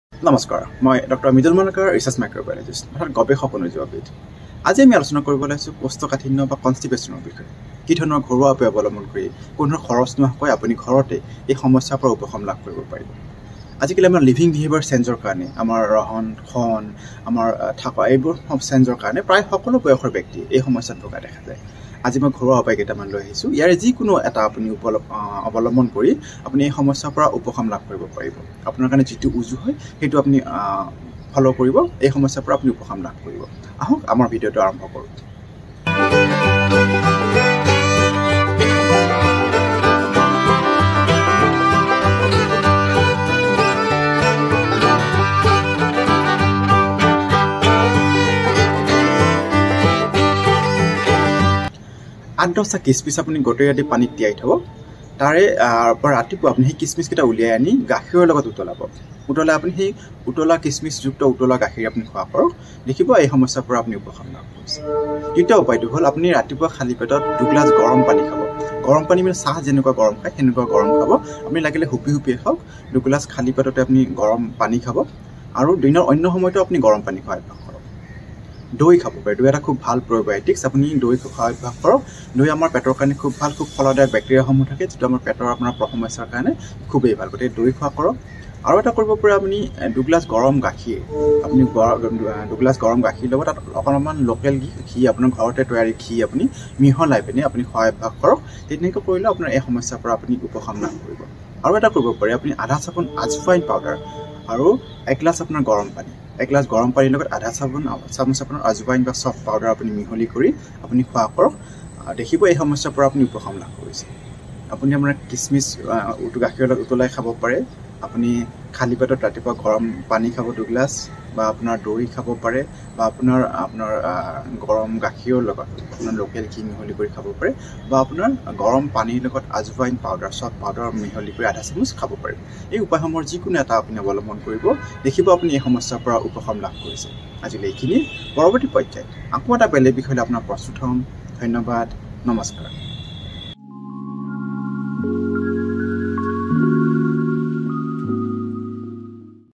পায়খানা কৰোঁতে বহু জোৰ দিব লাগে নেকি / constipation home remedies / পেট খোলোচা নহয় নেকি / ঘৰুৱা উপায়
This video describe about কোষ্ঠকাঠিন্যনিৰাময়ৰবাবেঘৰুৱাউপায় কিকৰিৱনহবলৈ হলেকিকৰিৱ ঘৰুৱাউপায় সাবধানহওক constipationhomeremedies healthtips healthadvice inAssamese healthadvice HealthyAssam Healthadvice Healthy_Assam in assamese

describe by Dr. Mridul Malakar
District Microbiologist
Kamrup-Metro
Assam
N.B:- This is the request to all viewers to follow the tips in addition with your's physician advice or with consultation with your treating physician 🙏.